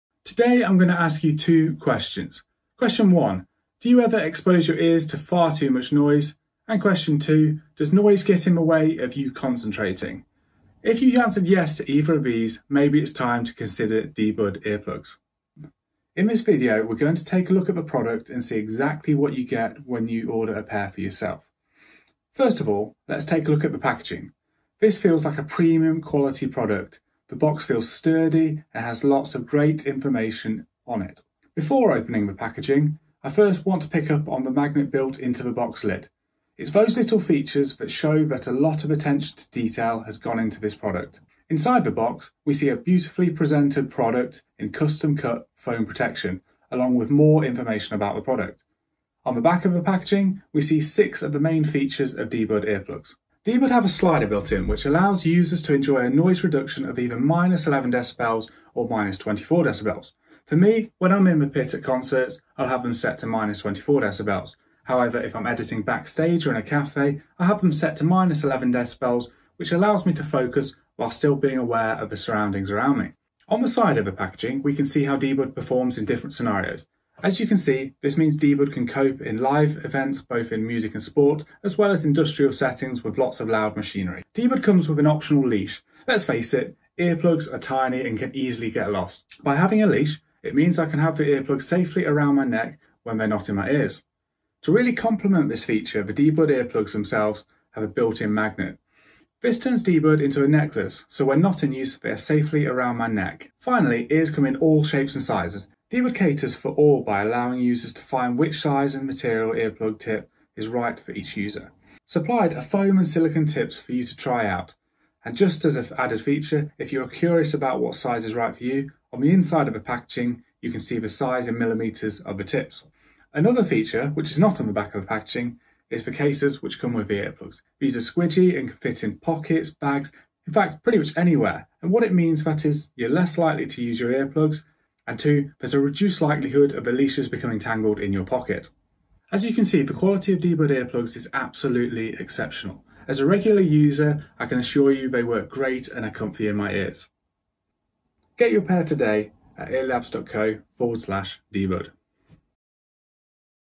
dBud in detail - unboxing
Looking at the quality and care of dBud earplugs.
Collaboration